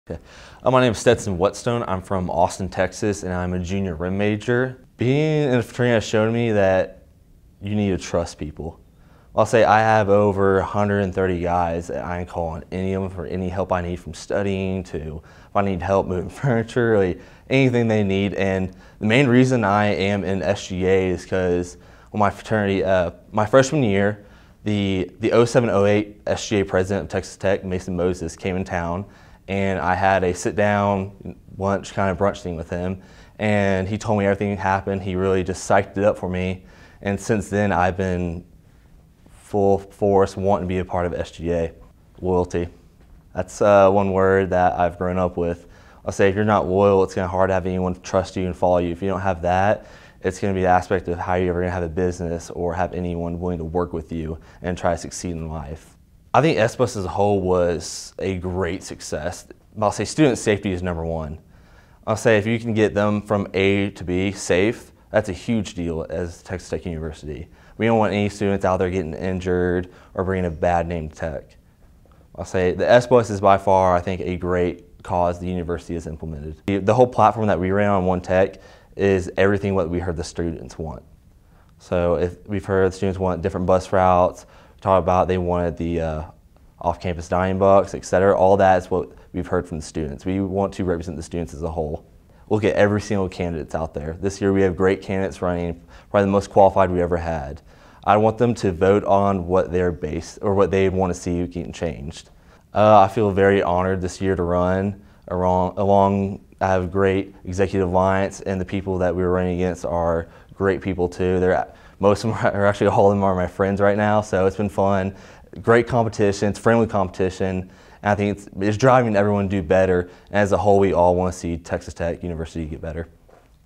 Stetson Whetstone - One Tech
One Tech's External VP candidate Stetson Whetstone talks about his plans for SGA.